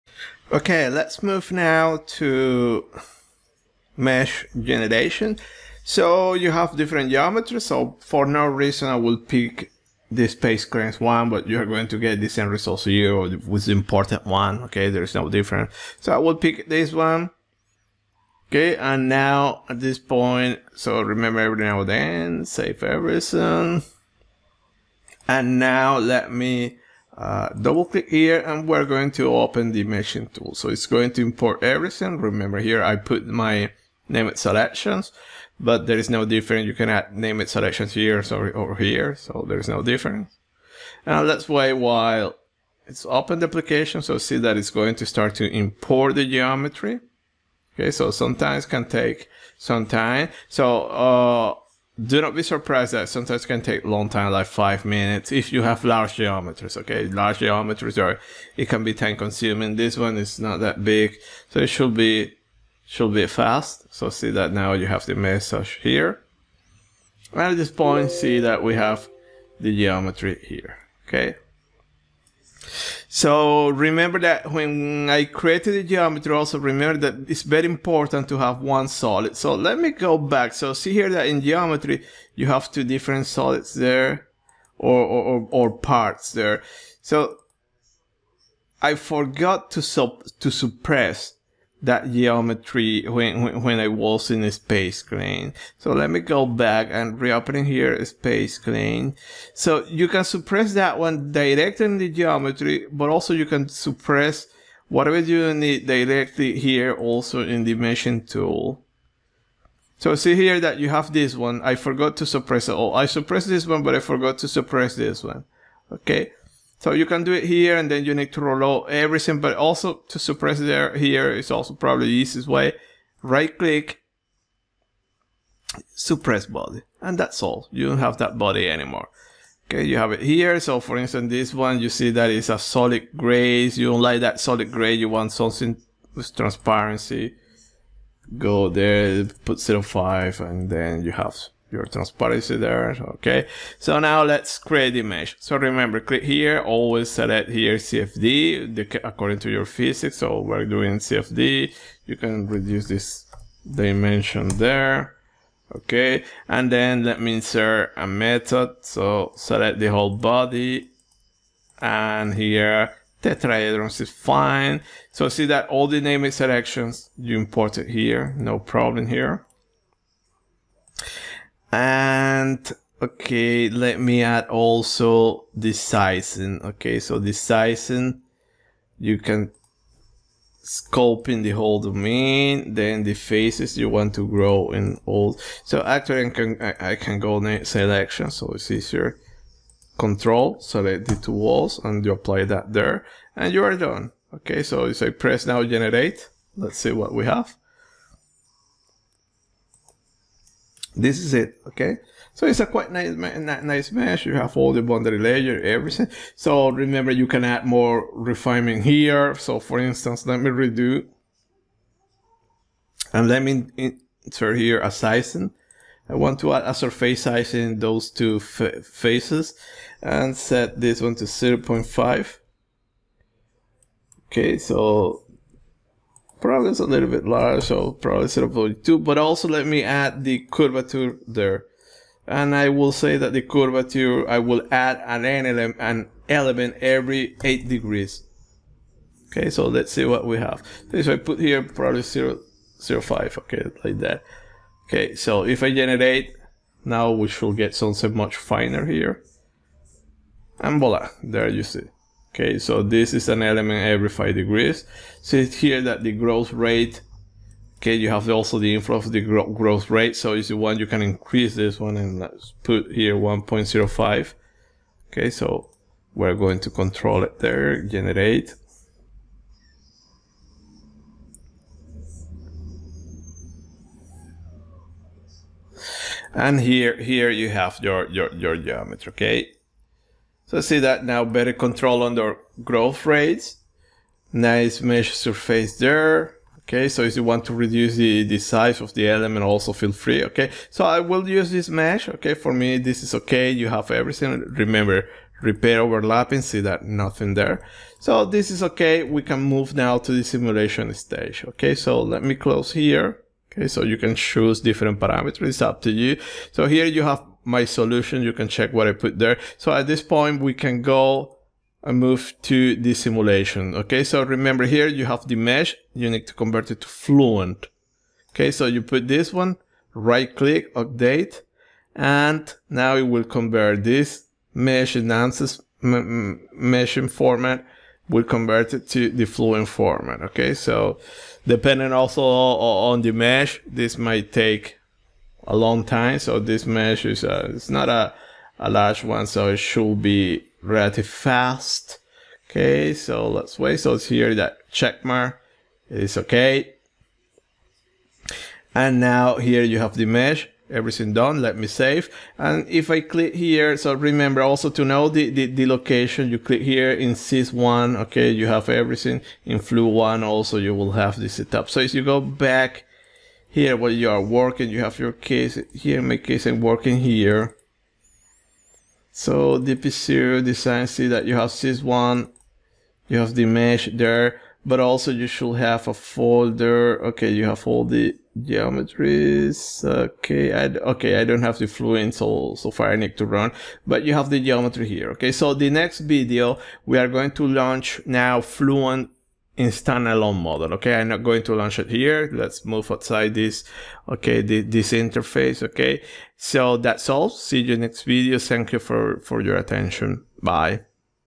Tutorial 4 - Part 4 - 3D pipe elbow.
UNIGE 2021 - Turbulence and CFD Models course

Turbulence modeling using Fluent
Tutorial 4. 3D pipe elbow.
Part 4. Ansys workbench - Mesh generation.

2D pipe elbow.
- Geometry generation.
- Mesh generation.
- Case setup in Fluent stand-alone.
- Basic post-processing.
- Adaptive mesh refinement